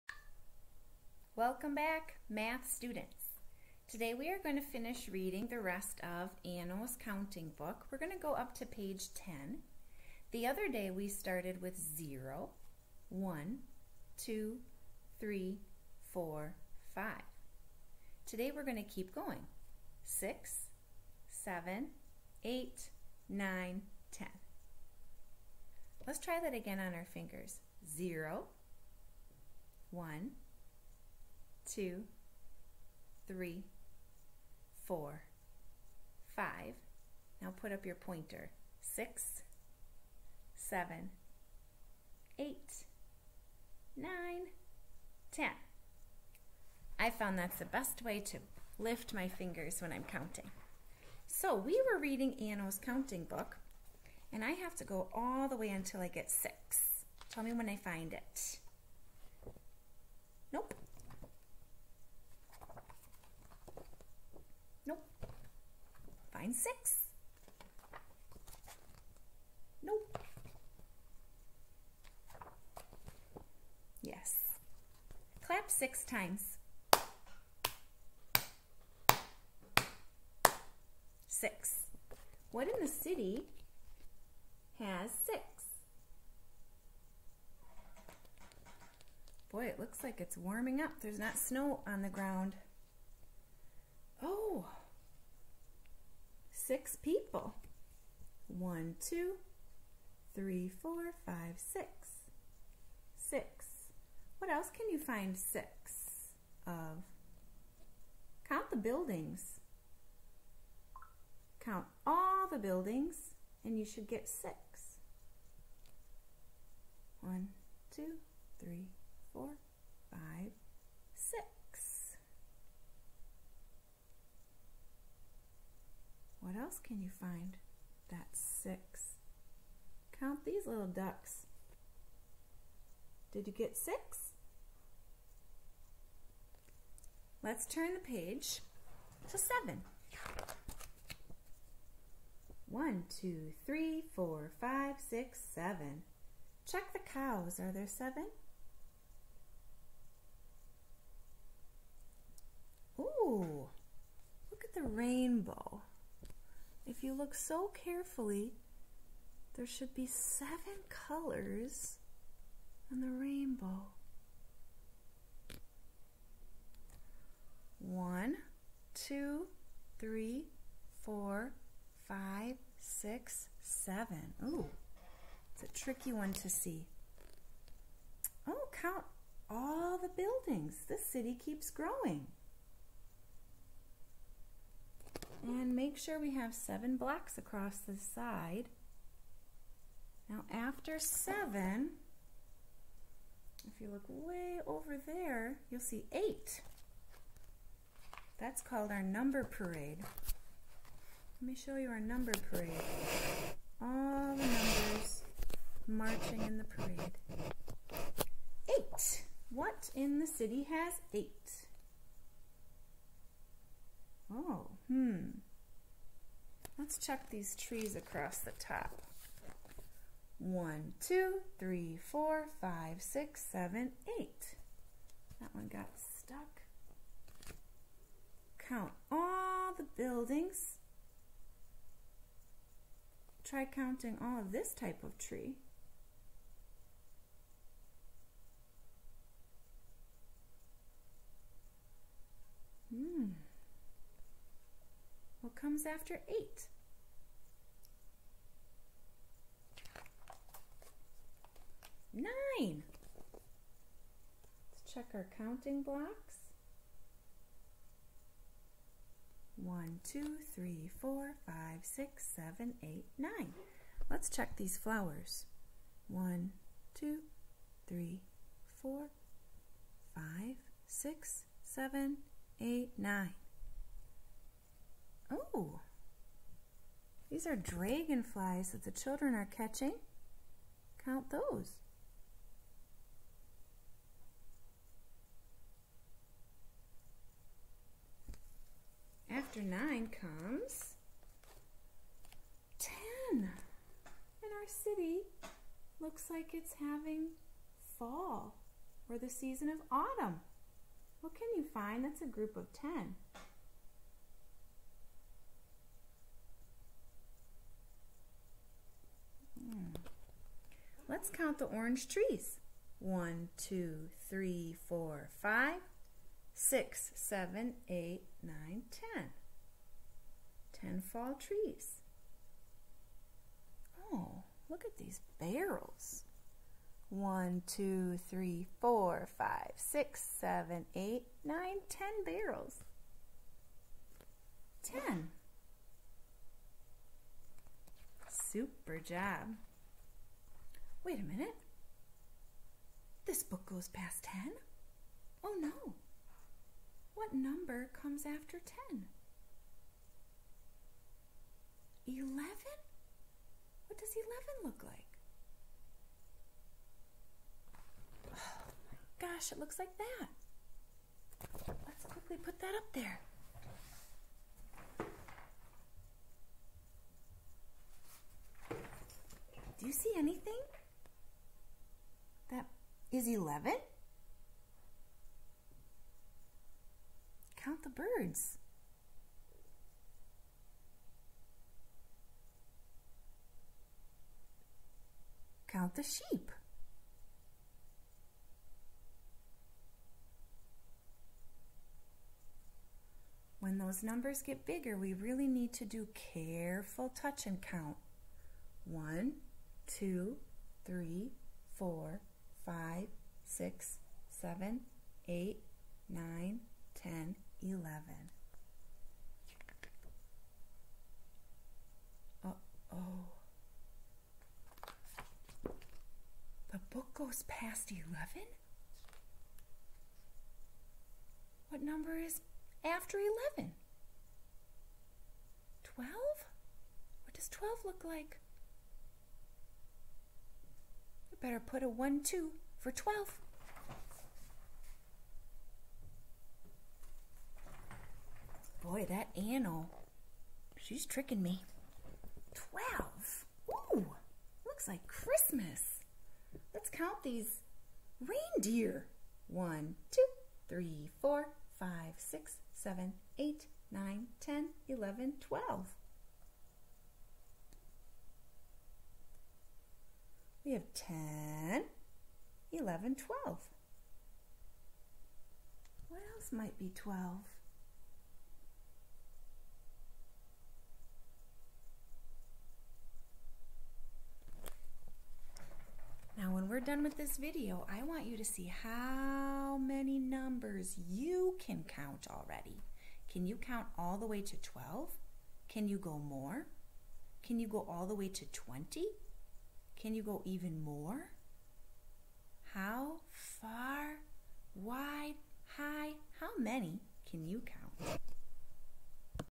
Anno's counting 6 to 12
Let's read the rest of the book.